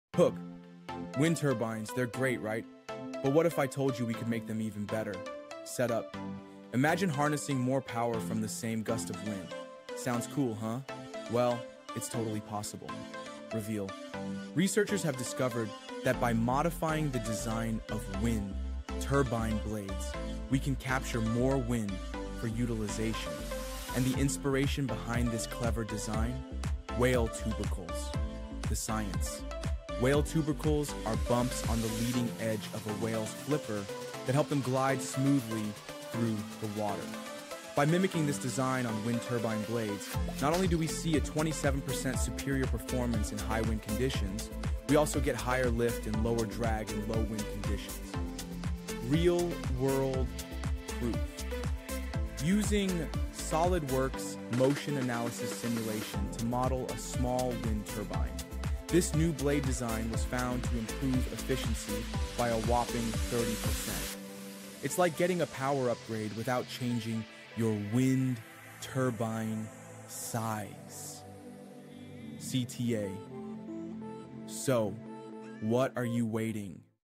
Get the Most Out of Your Wind Turbine with These Upgrades
Paper title: Design and Optimisation of Horizontal Axis Wind Turbine Blades Using Biomimicry of Whale Tubercles
Authors: John Yirijor and Nana Asabere Siaw-Mensah